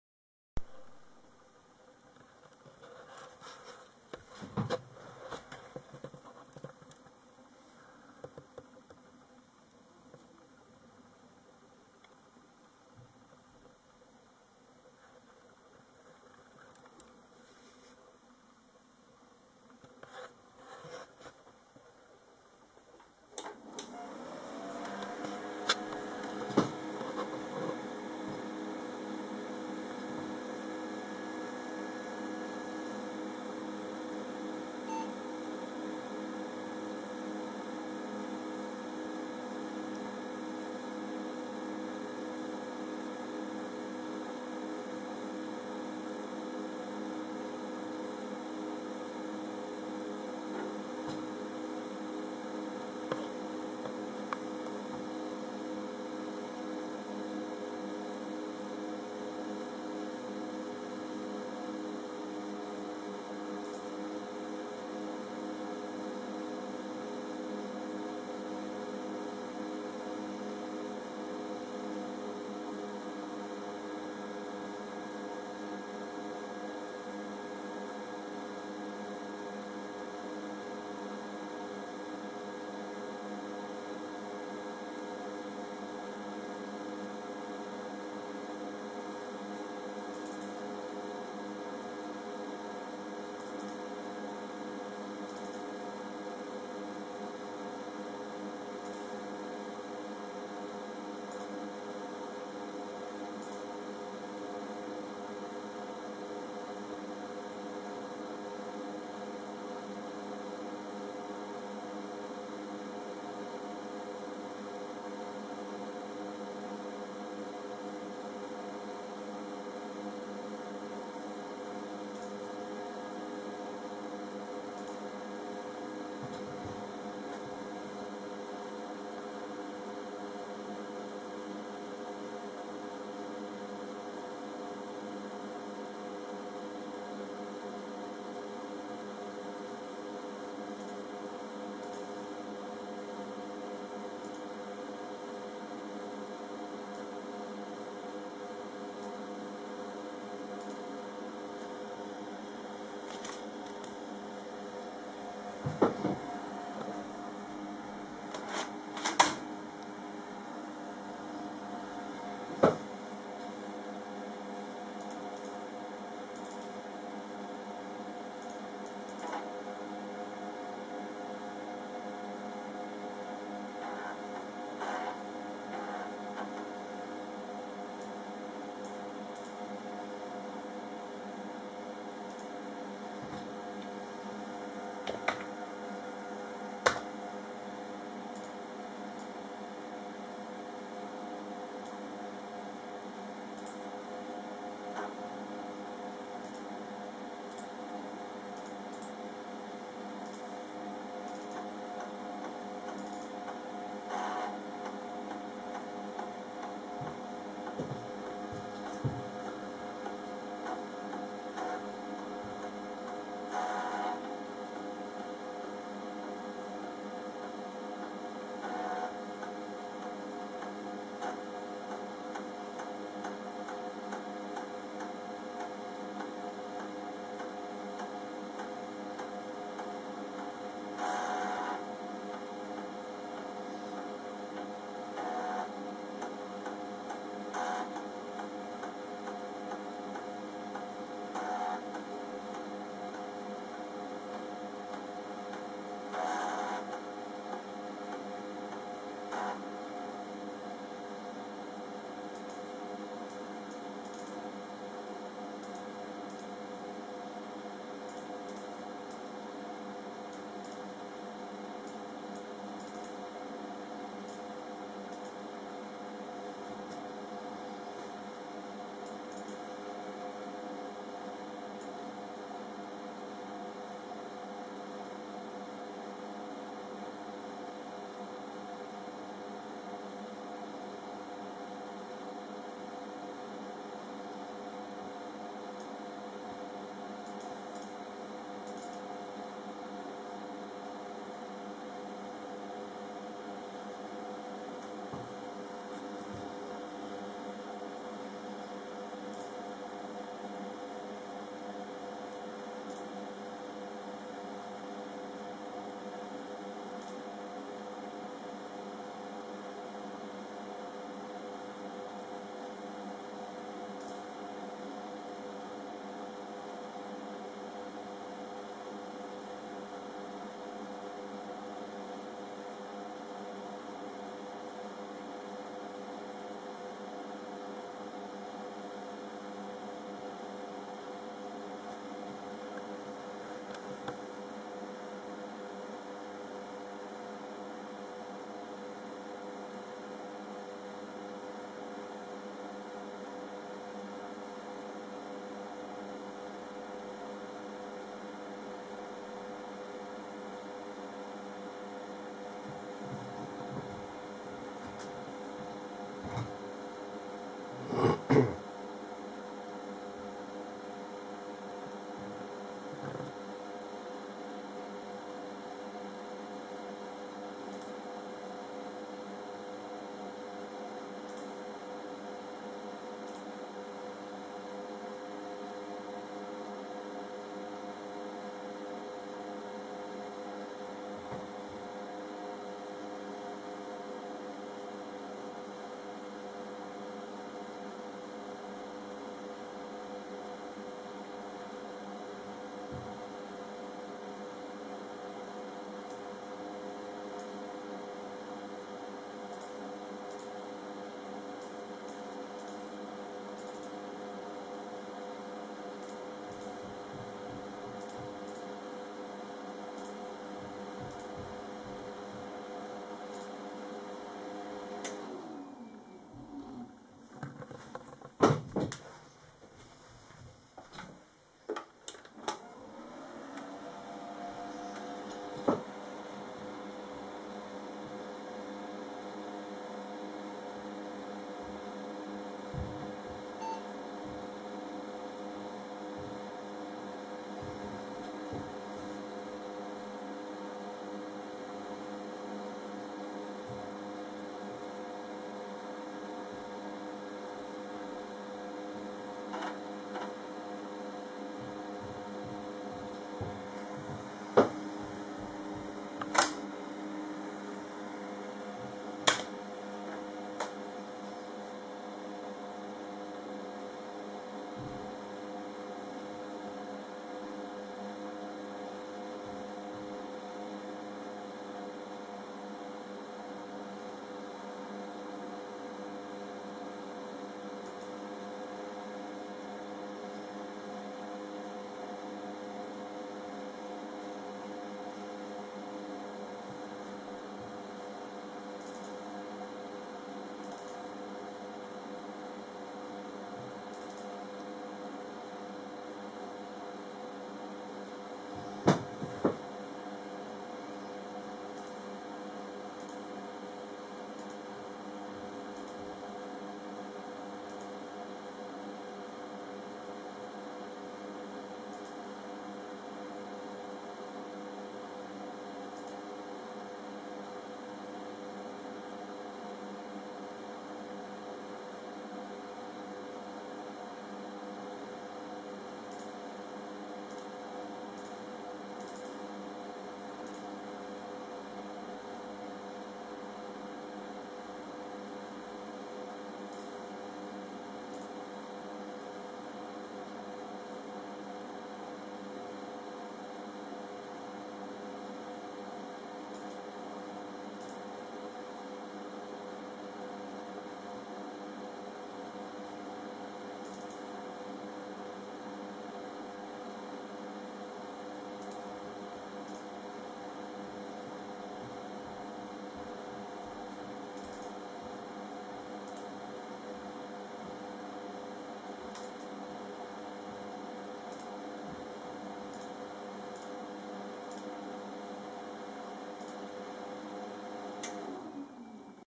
TB Multisound test
Testing of my Turtle Beach Multisound ISA card - First test since
storage several years ago! - Sadly didn't even show in 'control panel' / 'devices'. Software installed but showed unknown error (Hardware not initialised I guess)